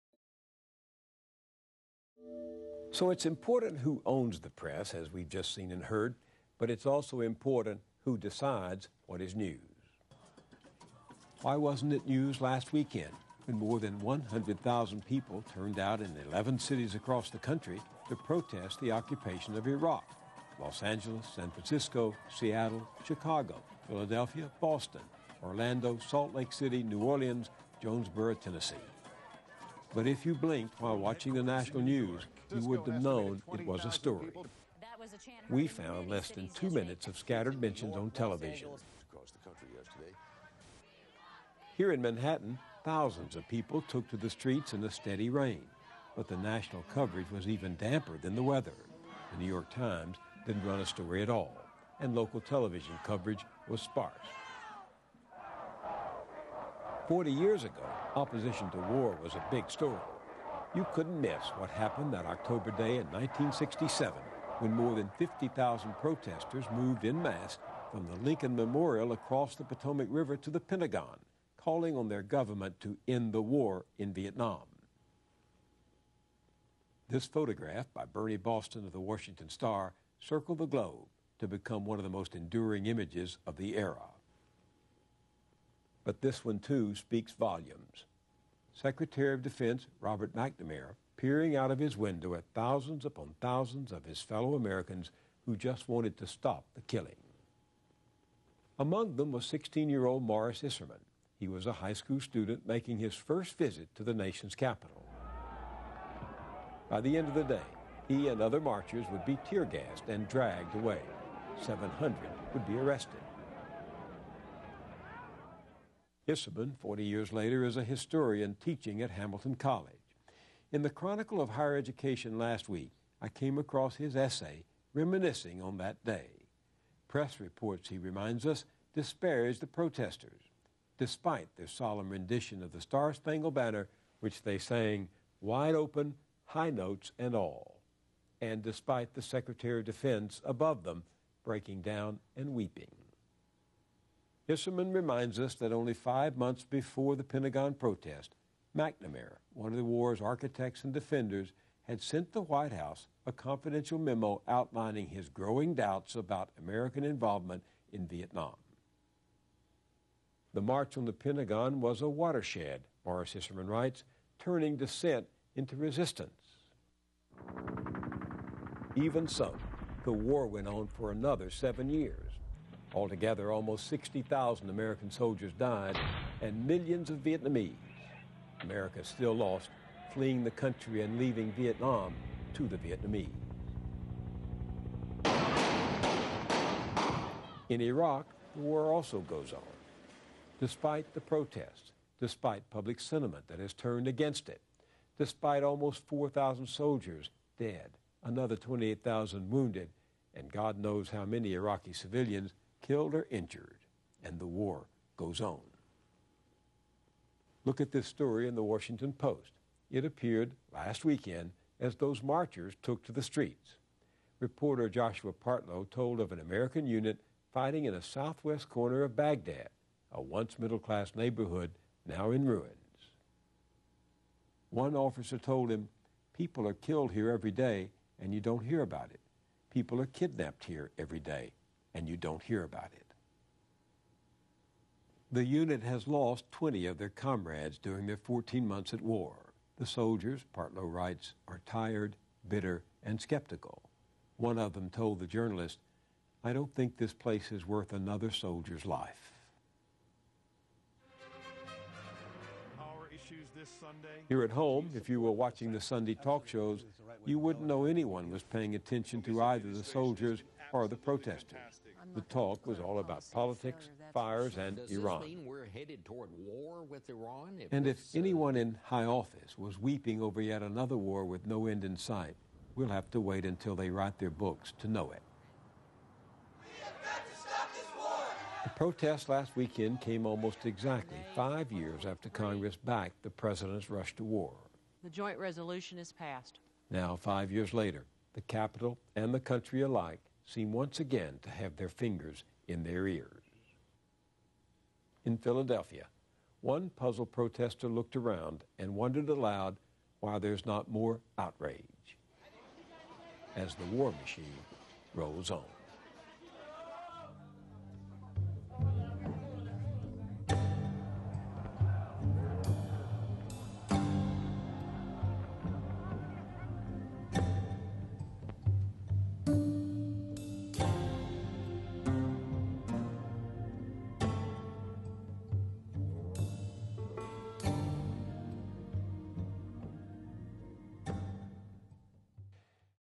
War Protests Actually Are News
Last weekend's war protests drew 100,000 people but they were nearly ignored by the mainstream media. Since the media are owned by some of the companies who actually benefit from perpetual war, it is no surprise that they would want people to believe there is no substantial opposition to the war. Public opposition to the war will spread when the public knows more about it.

Robert Kennedy Jr. said that media consolidation and campaign finance reform are two of the biggest issues we face today. Seek out independent media and vote for candidates who demonstrate independence from the special interests.